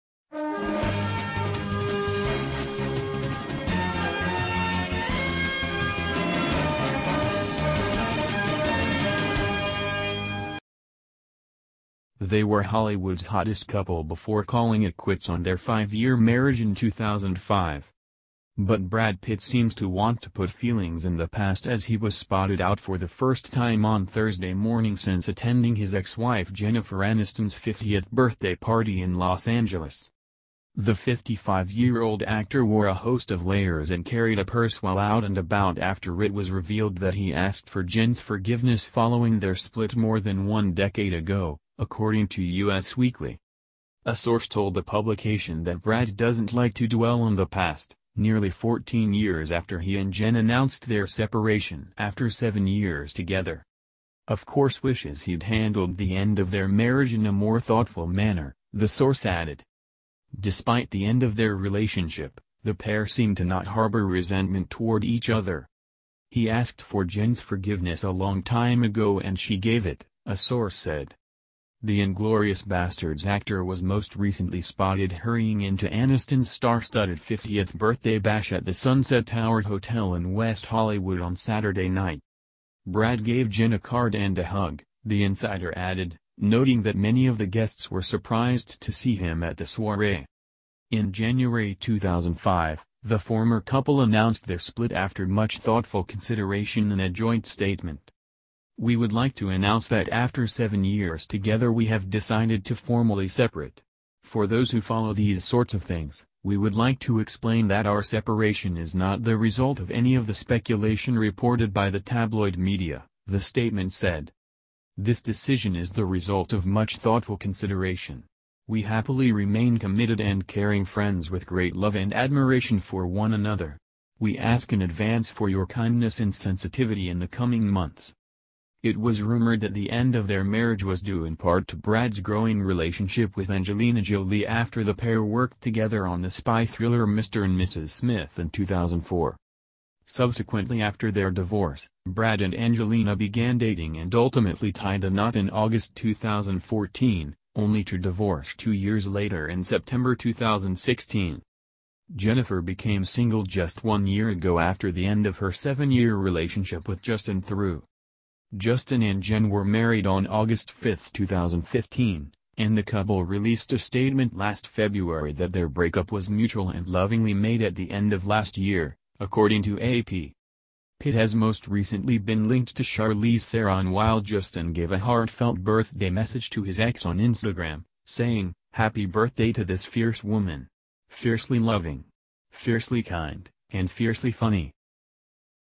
Brad Pitt asked for Jennifer Aniston's 'forgiveness' over how he handled their divorce
Brad Pitt emerges for first time since Jennifer Aniston's 50th birthday party... as it's revealed he asked for her 'forgiveness' over how he handled their divorce